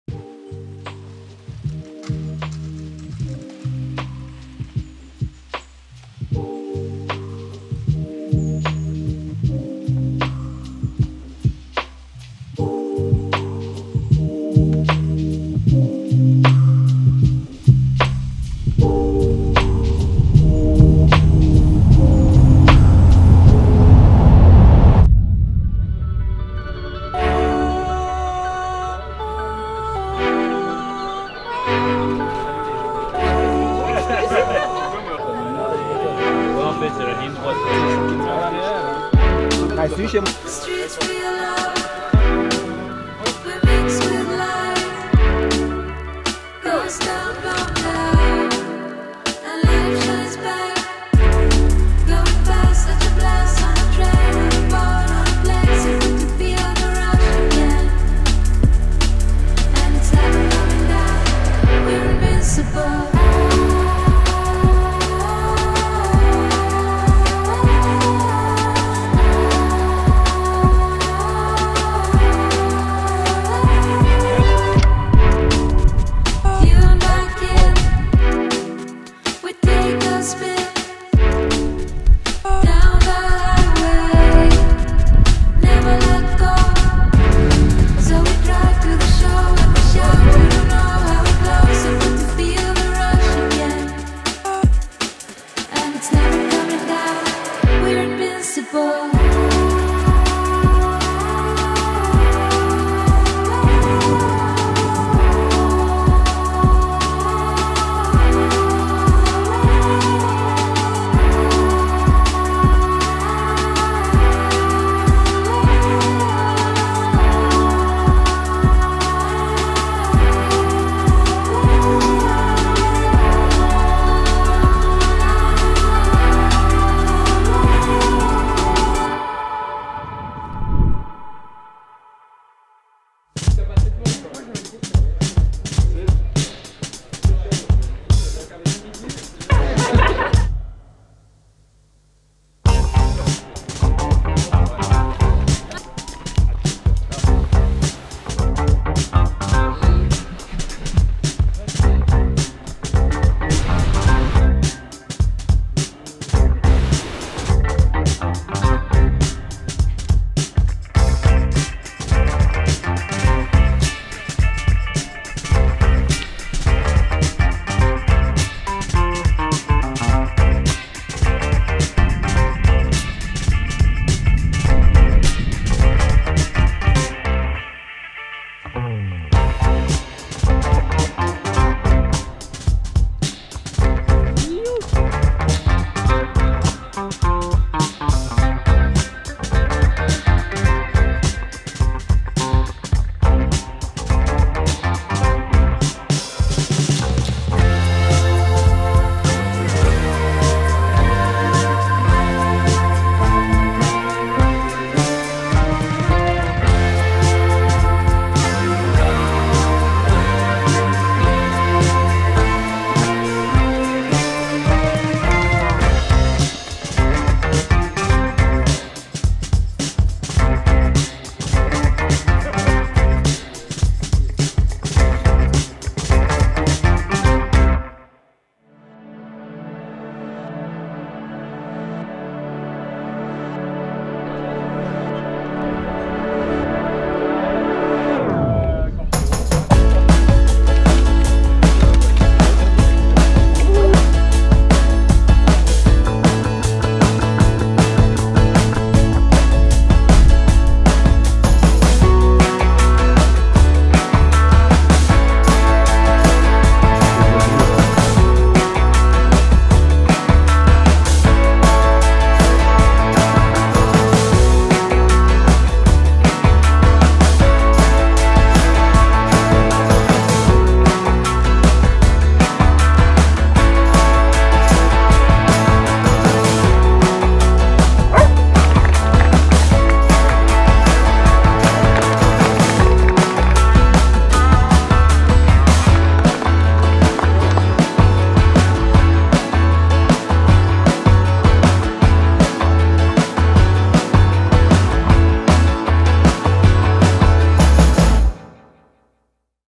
Jura Hike and Fly 2024
Paragliding hike and fly competition in the Jura from May 3rd to 5th 2024. - A film by Jolan Willenegger.